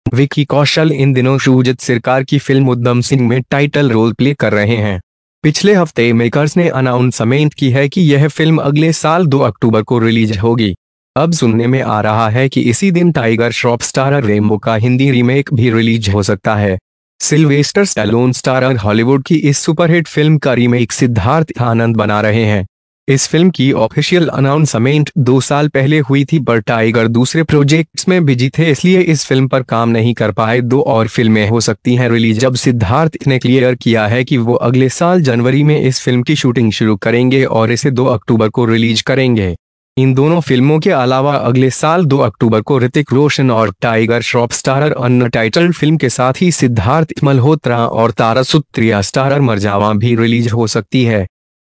टकराएंगी विकी कौशल और टाइगर श्रॉफ की फिल्में
Vicky Kaushal is playing titled role in Shujit Sircar's film Udham Singh these days. Last week Makers has announced that the film will be released on October 2 next year. It is now heard that the same day, the Hindi remake of Tiger Shroff Starr Rambo could be released. Sylvester Stallone Starter is remaking this super hit movie of Siddharth Anand The official announcement of this film took place two years ago but Tiger was busy in other projects, so the two films which could not work on the film could be released. Now Siddharth has cleared that he will start shooting for the film in January next year. And will release it on October 2. In addition to these two films, on October 2 next year, along with Hrithik Roshan and Tiger Shroff Starrer Untitled Film, Siddharth Malhotra and Tara Sutariya Starrer Mernajwah can also be released.